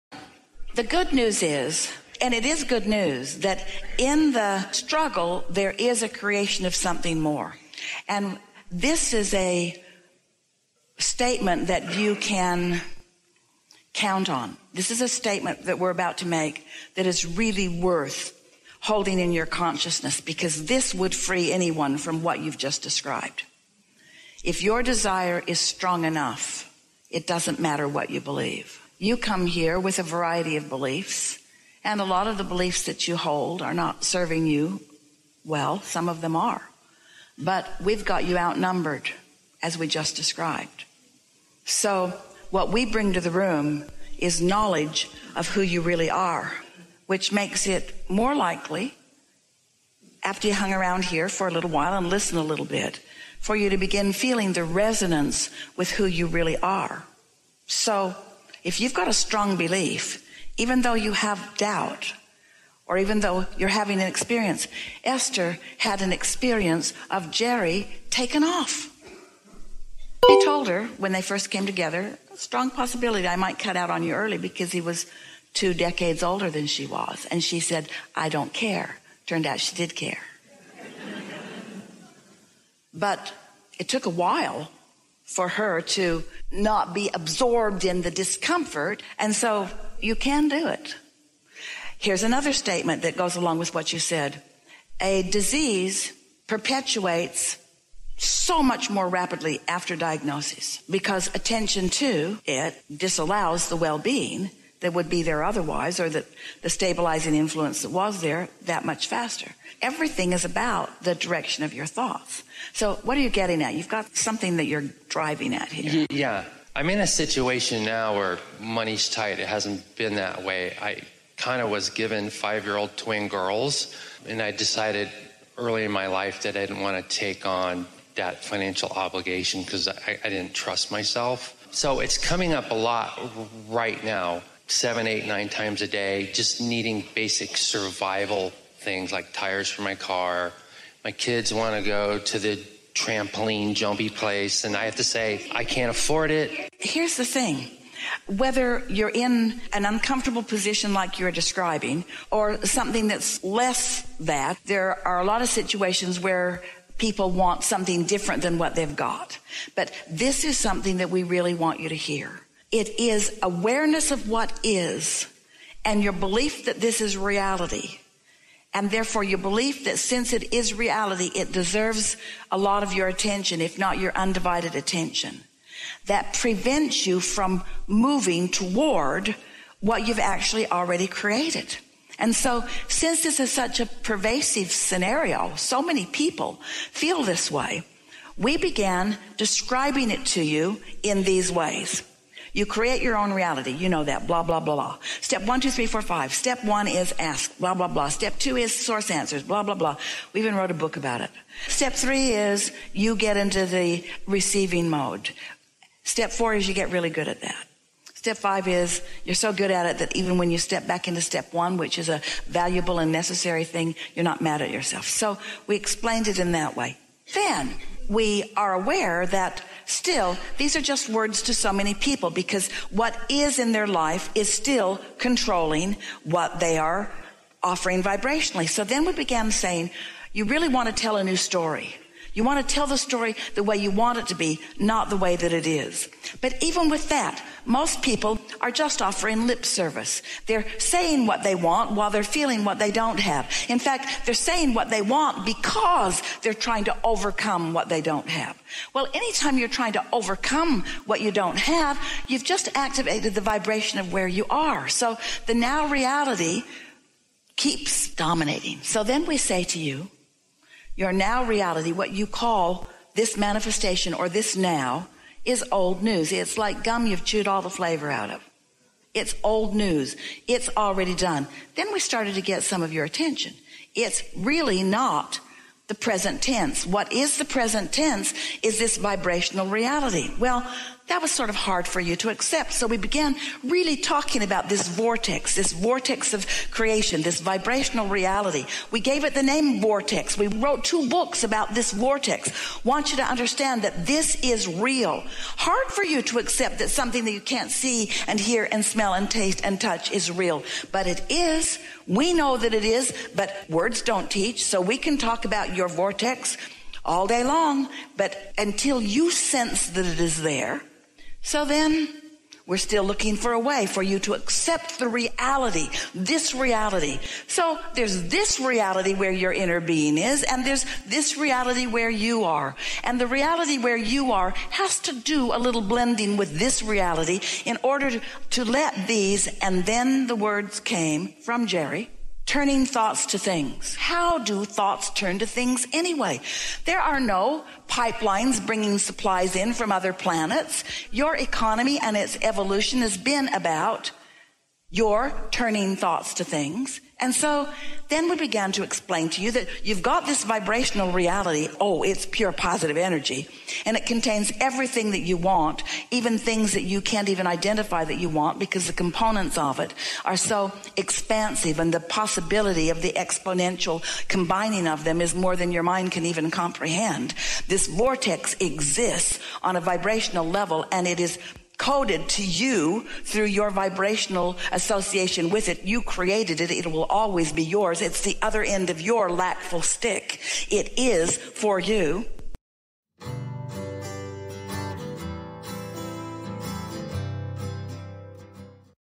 Thoughts turn to things!
Dive into the transformative teachings of Abraham Hicks with this enlightening video designed to elevate your life instantly! Join us on a journey of self-discovery and manifestation as we explore the profound wisdom of Abraham Hicks, renowned for their insights on the Law of Attraction. Learn how to harness the power of your thoughts and emotions to create a life of true joy and fulfillment.

Don't miss out on the opportunity to shift your mindset and embrace a new reality filled with abundance and happiness. Your journey to a truly wonderful life begins here!

All Abraham Hicks audio materials are copyrighted by Esther Hicks and we are sharing these following the USE OF COPYRIGHT GUIDELINES